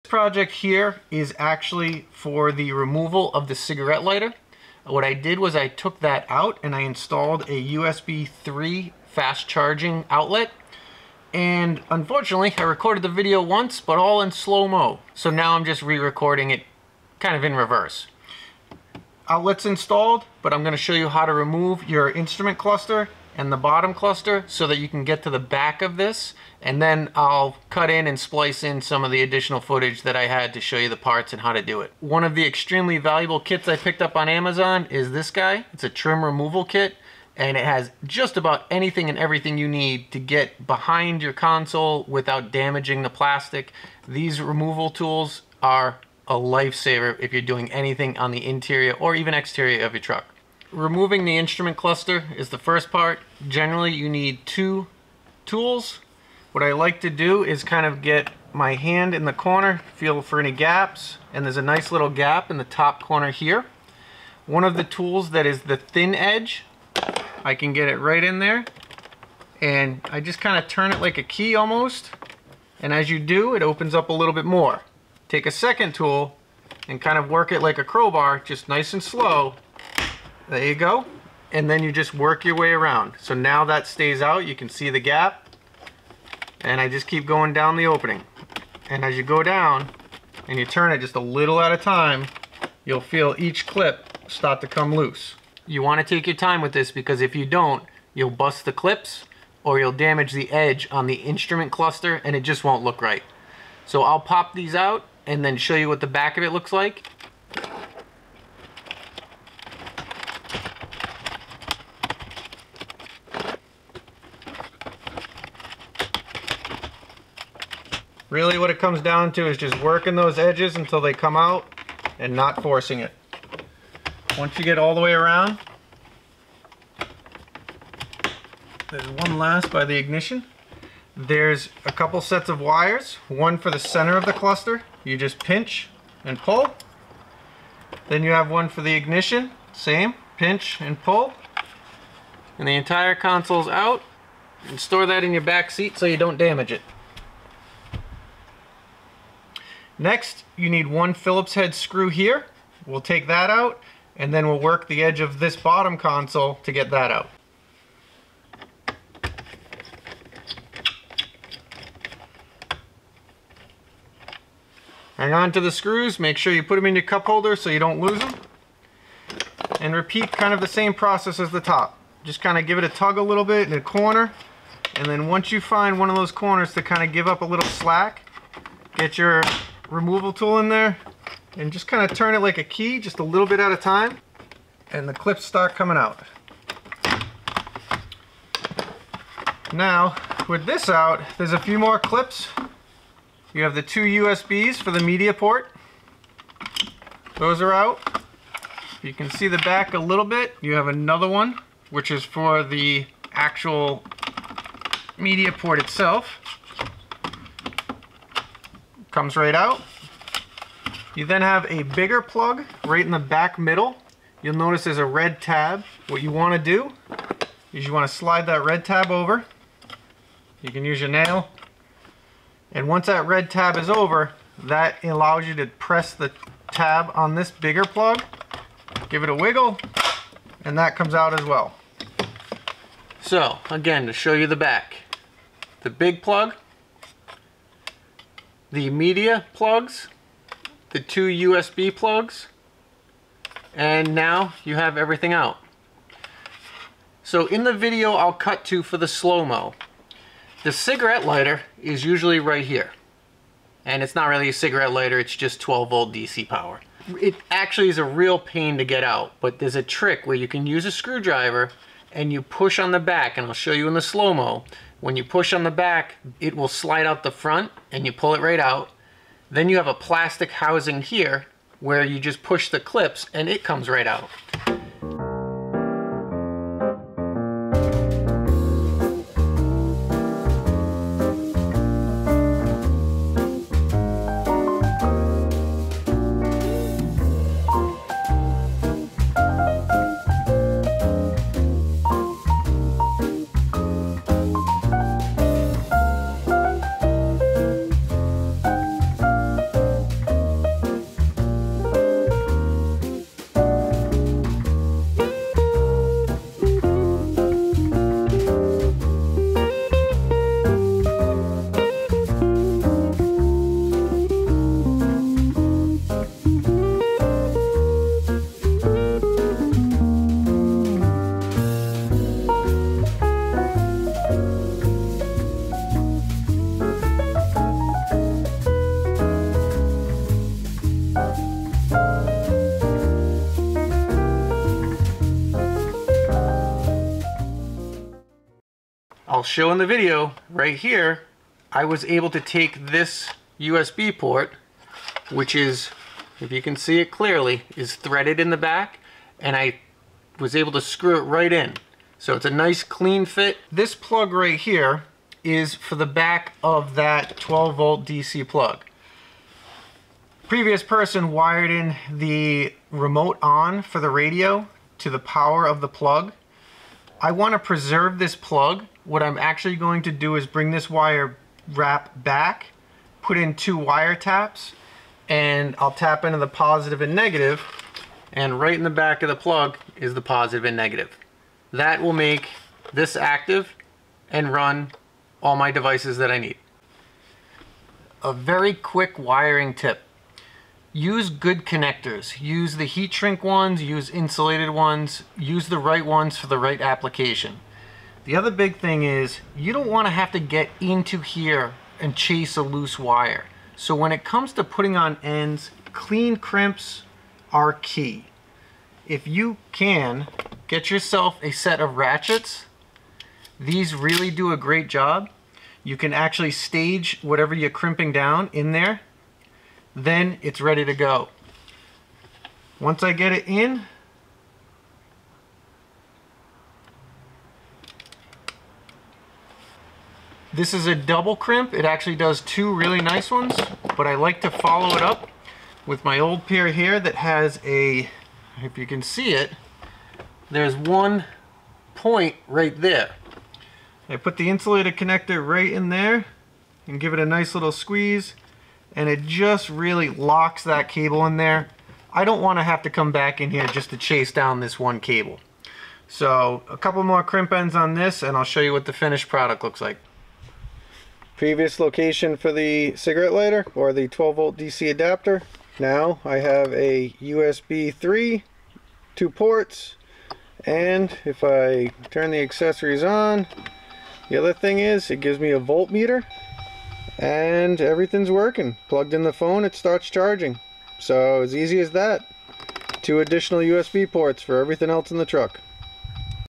Jeep Gladiator - 12c Dc outlet Swap for USB 3.0 Fast Charging
How to install a high speed 3.0 USB charger in your Jeep Gladiator.

Not my best video due to the fact my camera tried to sabotage me by recording everything in slow-mo and with no sound.

Pretty sure I was able to get most of the details when I re-recorded and hopefully it helps someone. If  you have any question please just reach out and I'll fill in any gaps if needed.

As always here are the links for items used.

- YONHAN 12V USB Outlet QC3.0 Dual USB Charger Socket
- GOOACC - GRC-49 19Pcs Trim Removal Tool Set
- Wiring Harness Cloth Tape 5 Packs
- Wirefy Crimping Tool For Insulated Electrical Connectors - Ratcheting Wire Crimper

00:00 Introduction
00:11 Project Intro
00:31 Must have Tool Kit
00:56 Remove Upper Instrument Cluster
02:15 Unplugging Cluster
02:41 Remove Lower Cluster
03:37 Unplugging Lower Cluster
04:10 SlowMo Remove of 12v DC Plug
04:35 Remove 12v Dc Outlet
05:13 Install Usb Outlet
05:40 Screw in USB Port
05:57 Using 12v DC Power for USB
06:29 Wire Crimping Tip
07:41 Finished Product